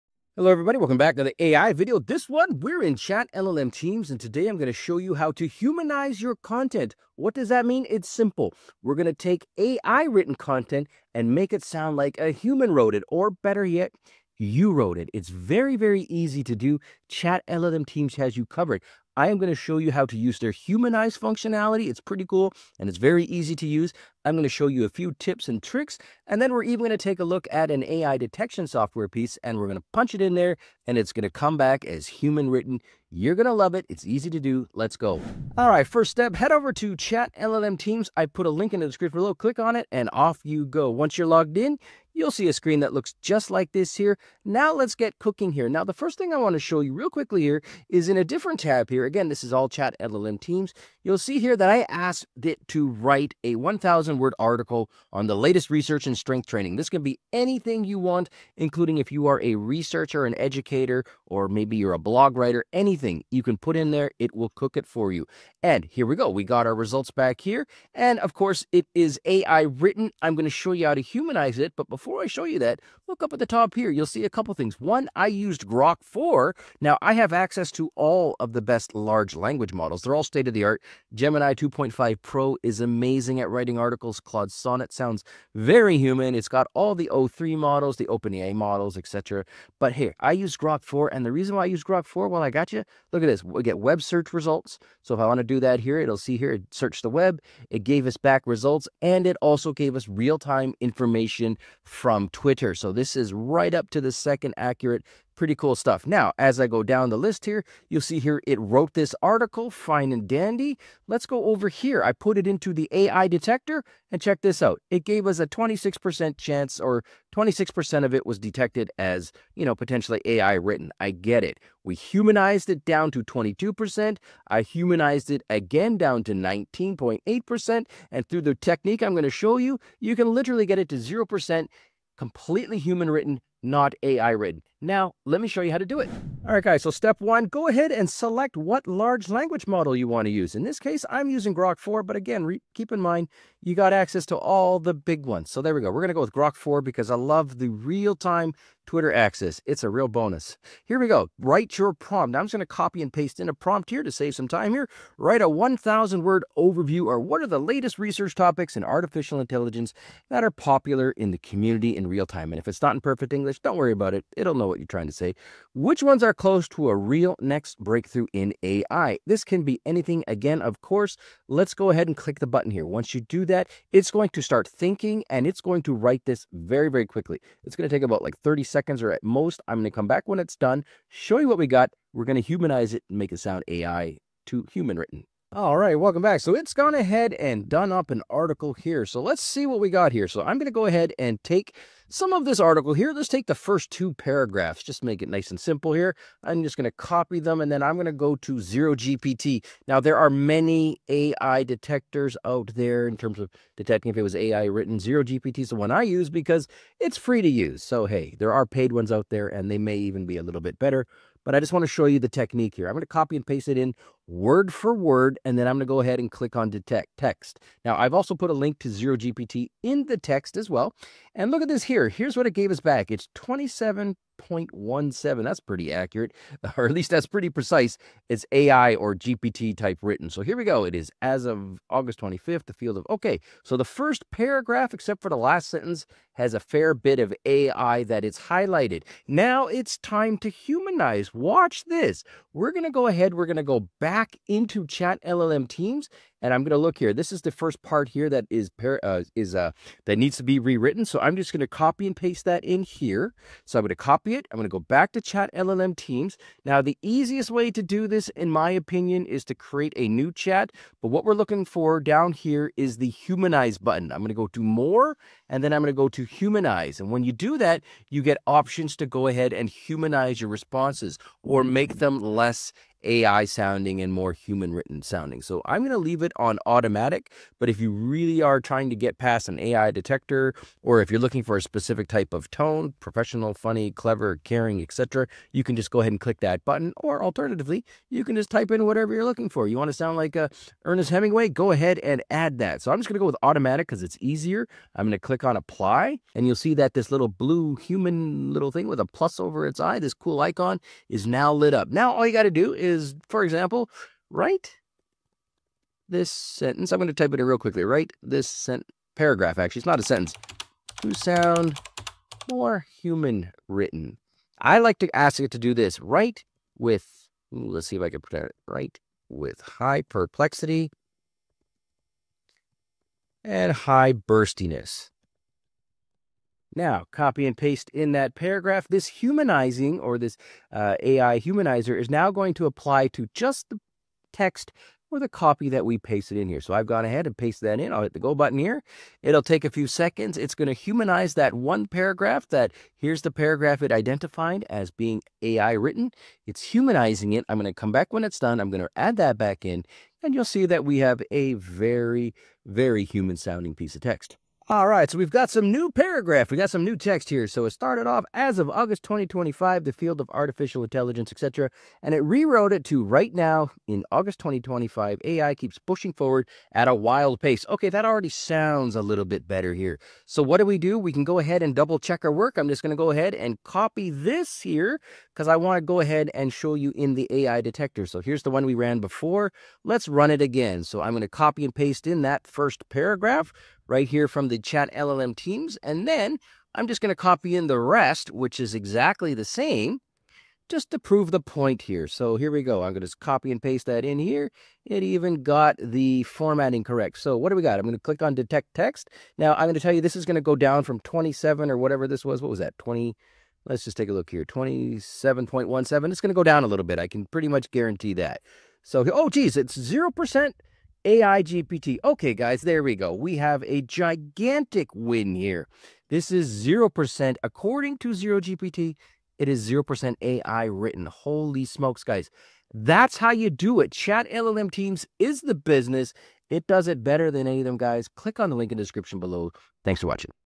Don't use ChatGPT for writing, it's easily detected - Use This Instead
Discover how to transform AI-generated text into human-sounding content that flies under the radar of AI detectors! In this quick tutorial, we dive into ChatLLM Teams by Abacus.AI and its powerful "Humanize" feature. Whether you're a blogger, researcher, or content creator, learn step-by-step how to make your AI-written articles sound like they were crafted by a real person – and even get them to score 0% on detectors like ZeroGPT.

We'll cover tips, tricks, and a live demo using Grok 4 for real-time web and Twitter searches to keep your content fresh and accurate. Perfect for anyone looking to humanize content without the hassle.

🔗 Get started with ChatLLM Teams: ChatLLM Teams
🔗 Try ZeroGPT AI Detector: ZeroGPT

Chapters:
0:00 - Intro to Humanizing AI Content
Welcome and overview of turning AI text into human-like writing using ChatLLM Teams.
0:17 - Features and Tips in ChatLLM Teams
Exploring the humanize functionality, tips, tricks, and a sneak peek at AI detection testing.
0:32 - Getting Started with ChatLLM Teams
Logging in, navigating the interface, and a quick example of generating AI content.
0:48 - Generating an AI Article Example
Asking ChatLLM to write a 1000-word article on strength training research.
1:06 - Choosing LLMs and Real-Time Search
Using Grok 4 for web searches and Twitter insights to get up-to-date info.
1:20 - Testing AI Detection Before Humanizing
Running the AI-generated text through a detector and seeing the results (26% AI detected).
1:58 - Step-by-Step Humanizing Process
Selecting an LLM, writing a prompt, and generating real-time AI research overview.
2:52 - Reviewing Generated Content
Checking the AI-written article and preparing to humanize it.
3:09 - Using ZeroGPT for Detection
Pasting text into ZeroGPT and analyzing the 27% AI score.
3:59 - Humanizing Text in ChatLLM
Copying AI text, creating a new chat, and activating the Humanize tool.
4:35 - Customizing Humanize Options
Setting tone (automatic, professional, etc.) and applying it to your text.
5:09 - Rewriting with High Perplexity
Pasting the paragraph and letting Humanize make it sound more natural.
5:54 - Reviewing Humanized Text
Comparing the original and humanized versions for better flow.
6:12 - Retesting in ZeroGPT
Pasting the humanized text and seeing the improved score.
6:46 - Final Results and Win
Achieving 0% AI detection – proof that ChatLLM Teams nails it!
7:09 - Outro and Call to Action
Wrap-up and encouragement to try ChatLLM Teams.